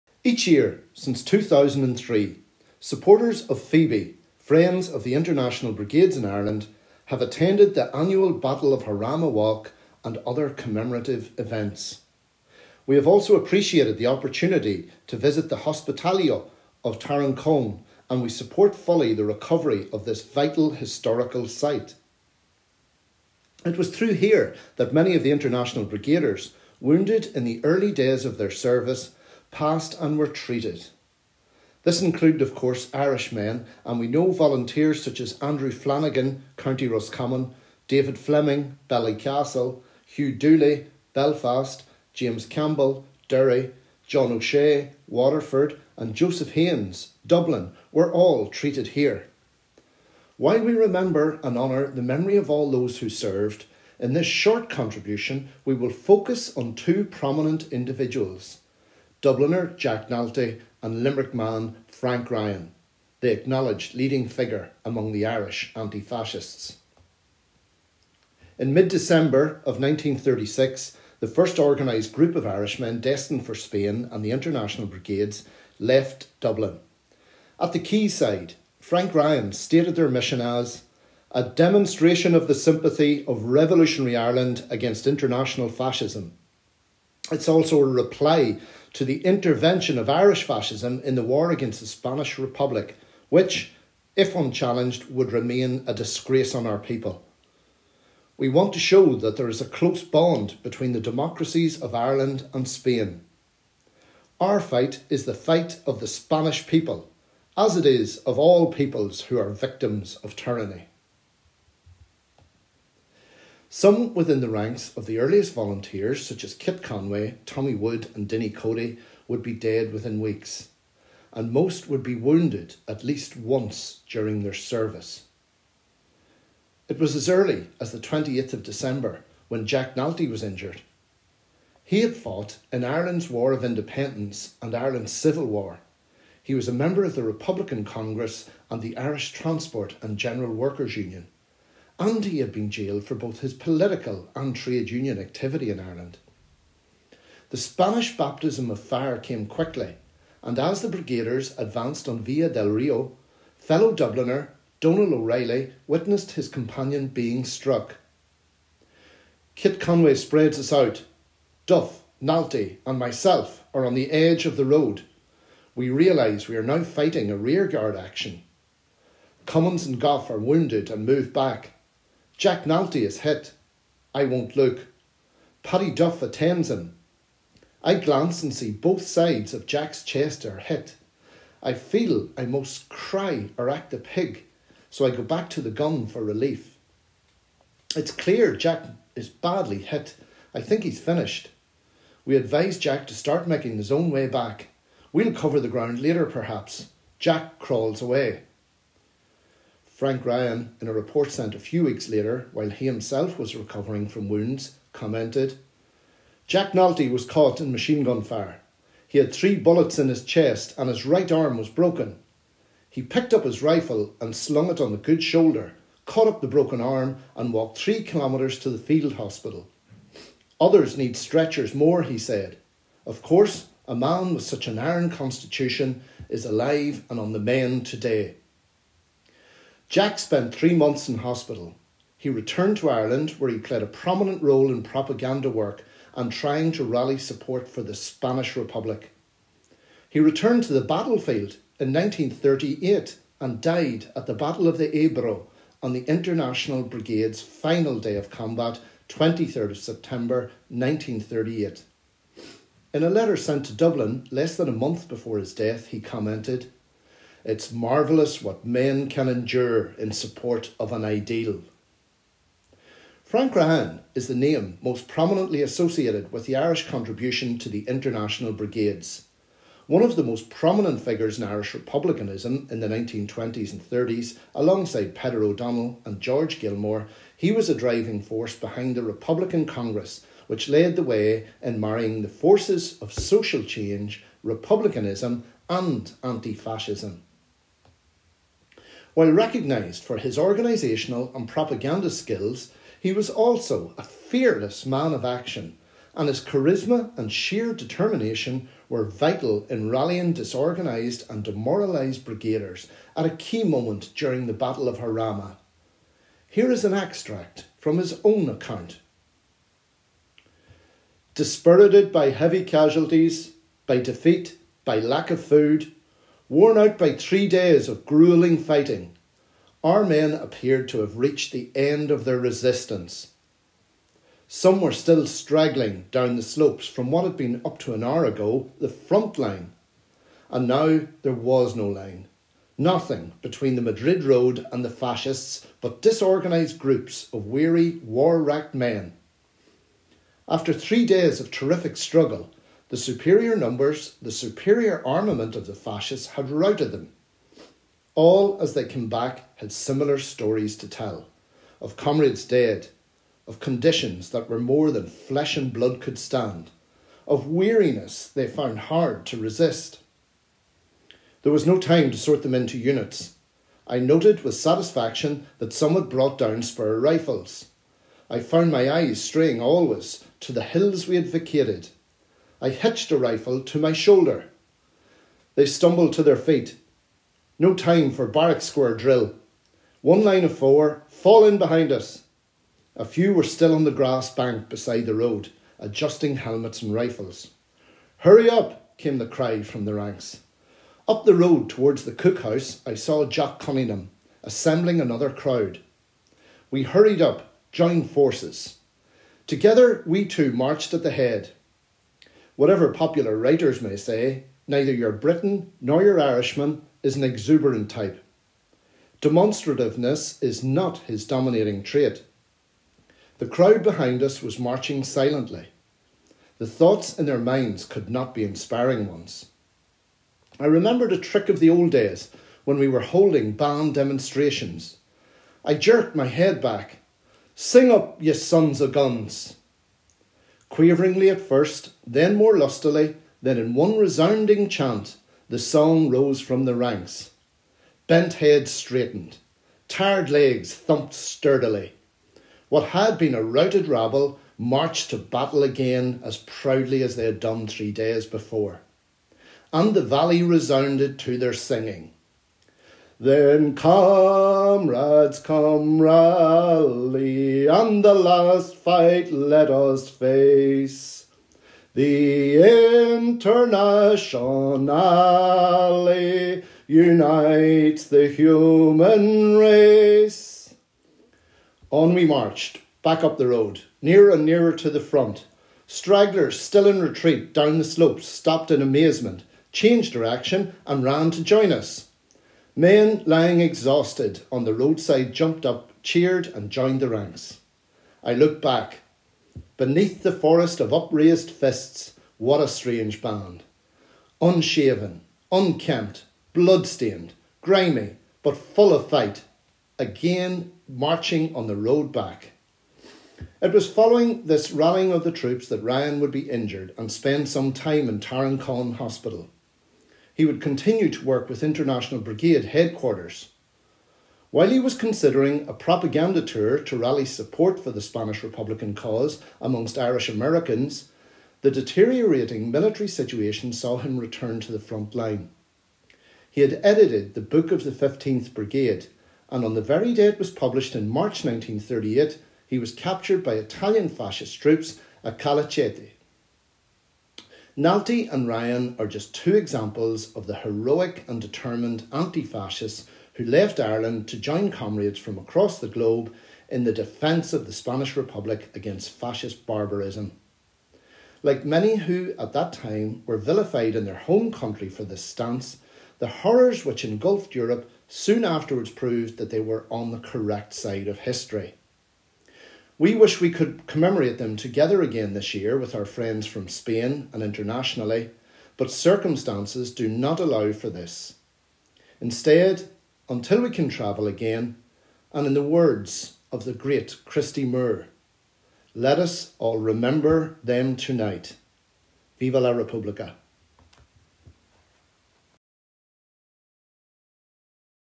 Battle of Jarama and Hospitalillo de Tarancón - Solidarity video 2021
Each year since 2003 supporters of FIBI , Friends of the International Brigades in Ireland have attended the annual Battle of Jarama walk and other commemorative events . We have also appreciated the opportunity to visit the 'Hospitalillo' of Tarancón and we support fully the recovery of this vital historical site.

This February we had hoped to join together again  with our friends from Spain and Internationally, but the circumstances of the COVID-19 pandemic did not allow for this.

This short video was produced to share with our friends and comrades , and hopefully we will all be together again soon.